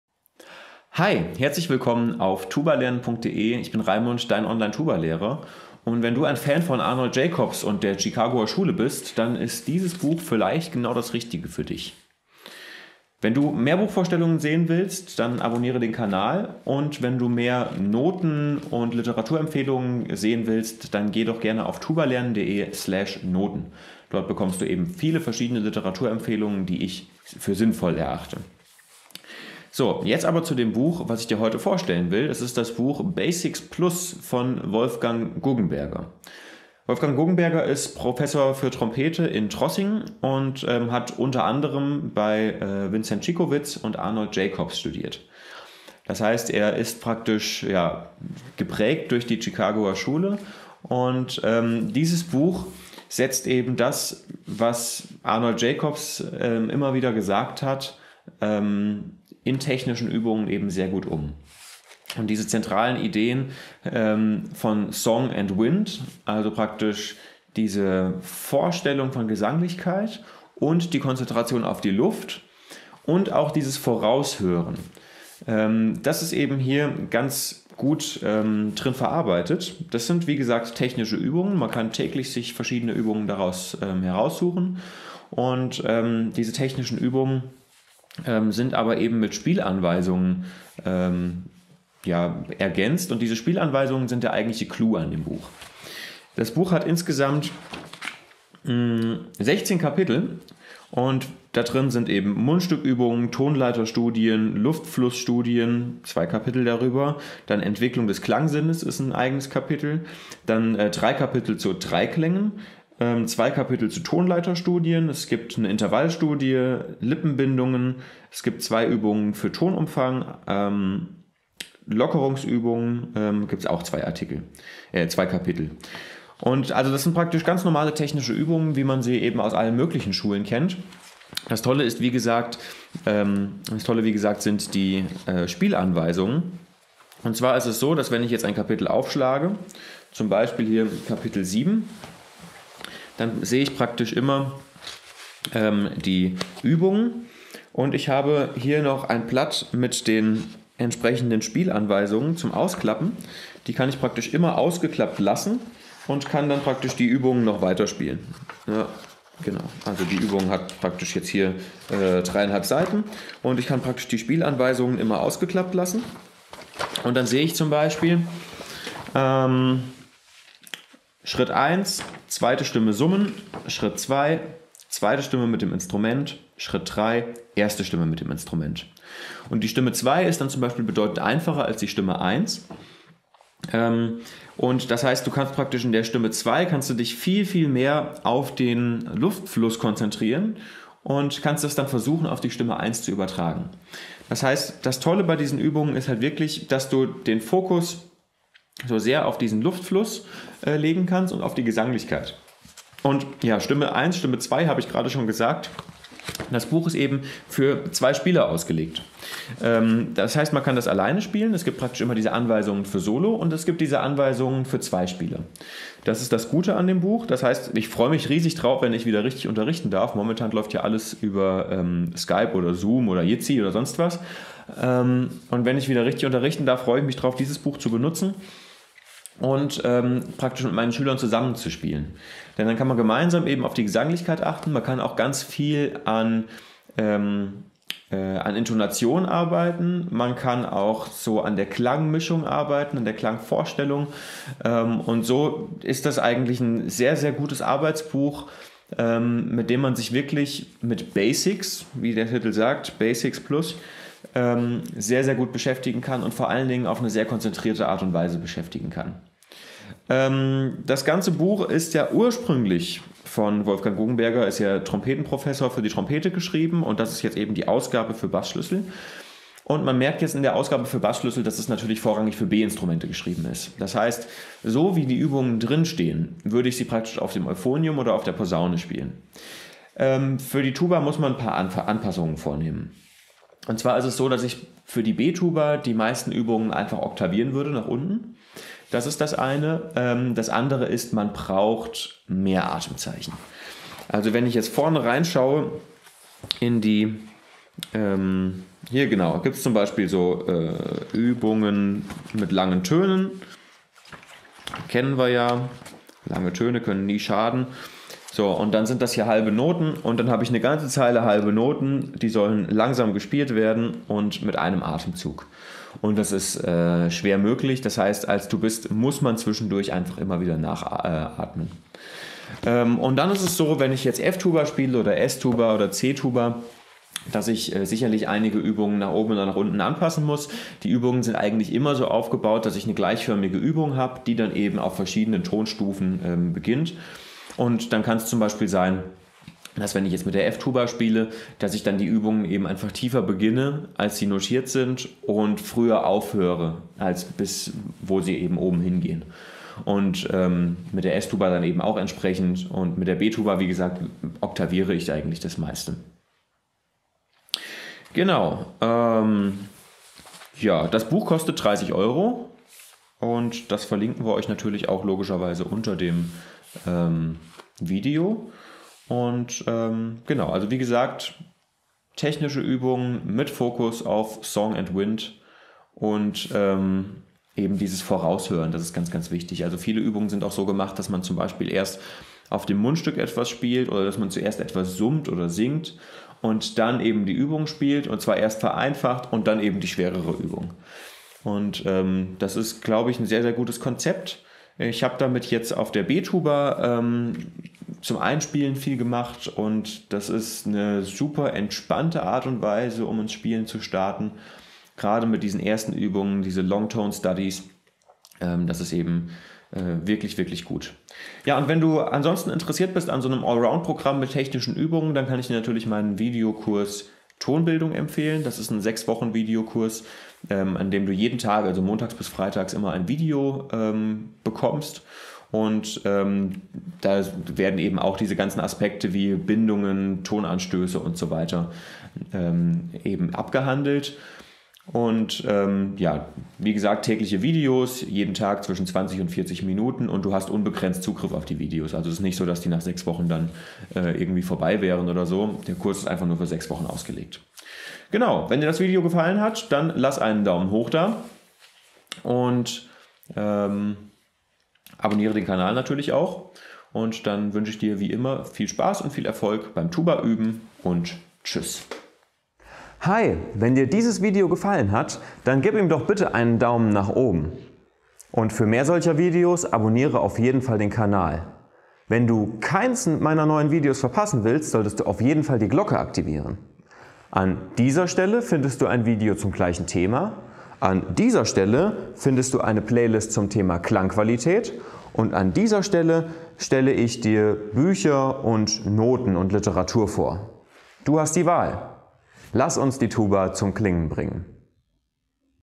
Buchvorstellung: "Basics Plus" von Wolfgang Guggenberger
'Song and Wind' war das Mantra von Arnold Jacobs. In dieser Tradition steht der Trossinger Trompetenprofessor Wolfgang Guggenberger, der mit Basics Plus Übungen für ein bis zwei Instrumente im Bassschlüssel vorlegt. Besonders sind dabei immer die Spielanweisungen, die die Standardübungen aufwerten, sodass du im Bereich der Tonvorstellung und des Luftflusses mehr aus den Übungen herausholen kannst.

Komm in die tubalernen-Community auf Facebook!